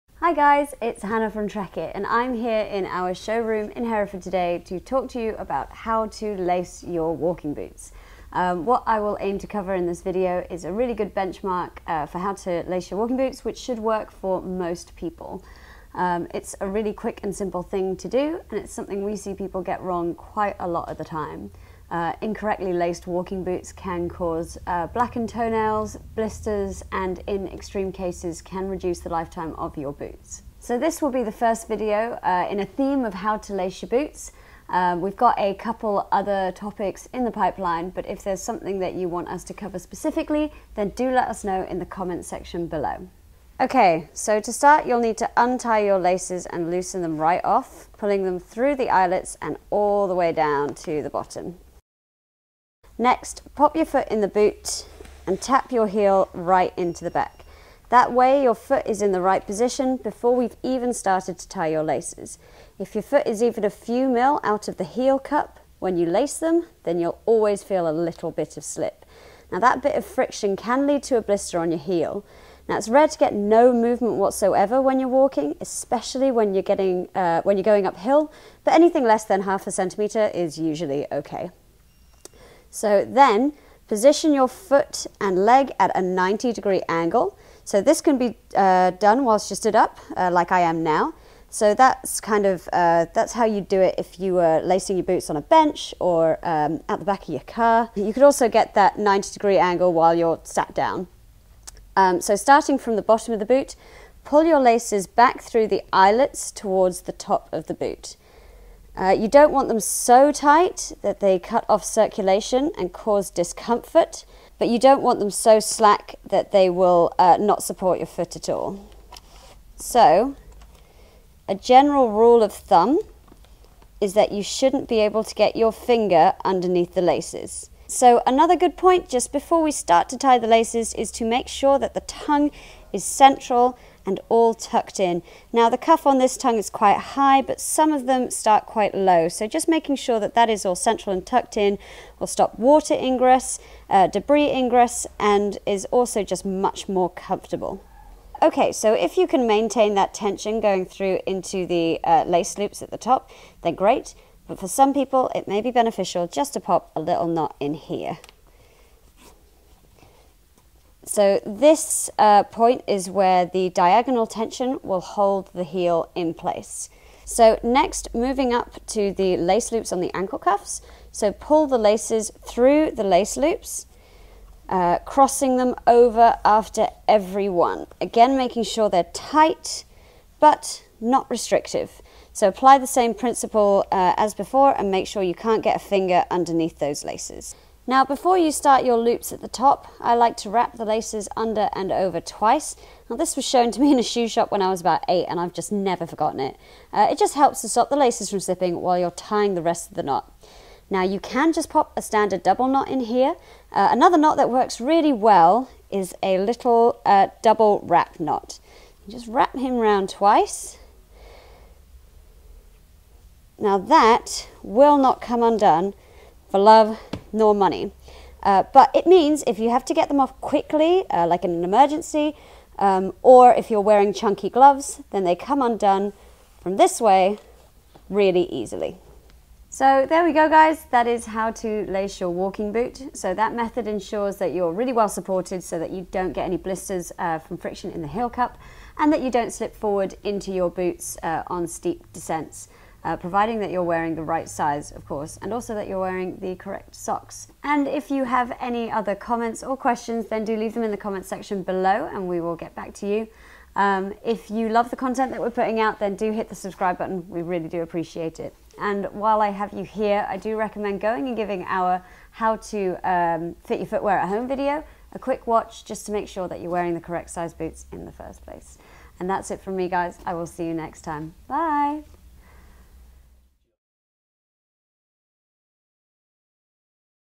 How to Correctly Lace Walking Boots
Despite it seeming like a simple task, lacing your boots can be the difference between an enjoyable walk in the mountains and black toenails. Are there any videos we've not done that you'd like to see? Let us know!